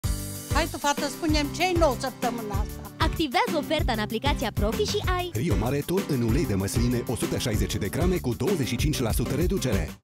Activează oferta în aplicația Profi și ai: Rio Mare, Ton în Ulei de Măsline 160g cu 25% reducere.
Activează oferta în aplicația Profi și ai: Rio Mare, Ton în Ulei de Măsline 160g cu 25% reducere. Oferta supusa unor termene si conditii. Profi,zilnic preturi mici.